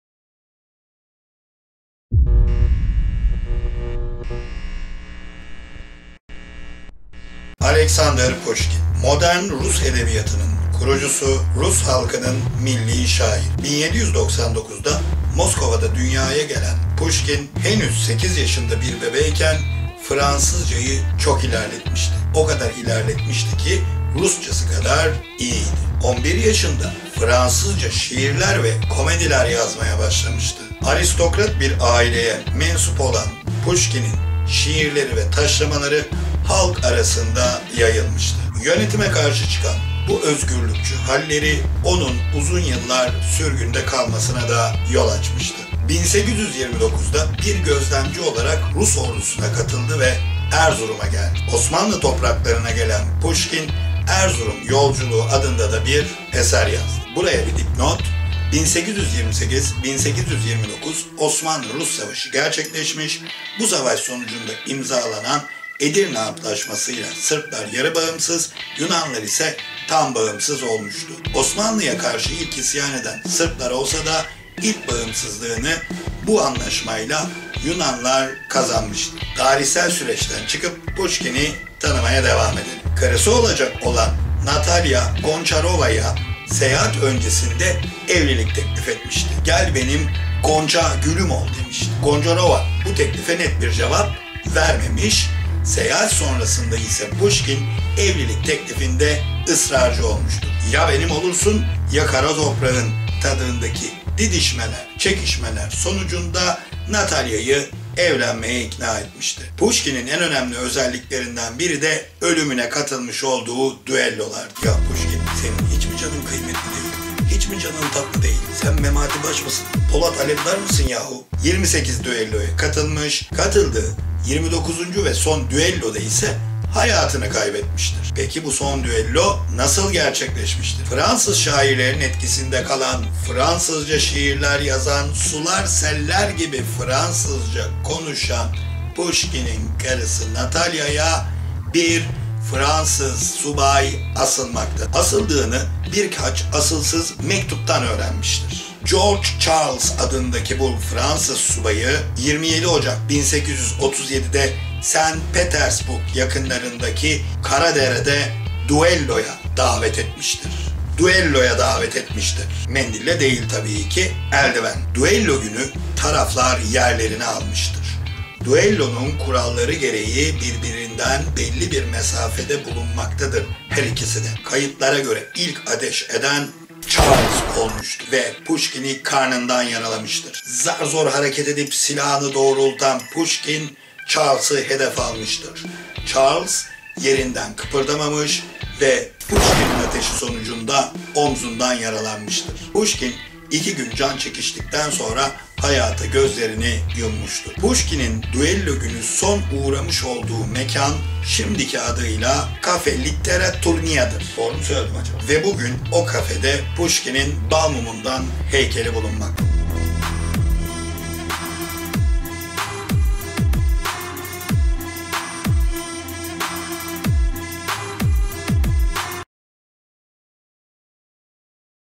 PİSİ PİSİNE GİDEN PUŞKİN
MODERN RUS EDEBİYATININ KURUCUSU PUŞKİ'İN ŞAŞIRTAN SONU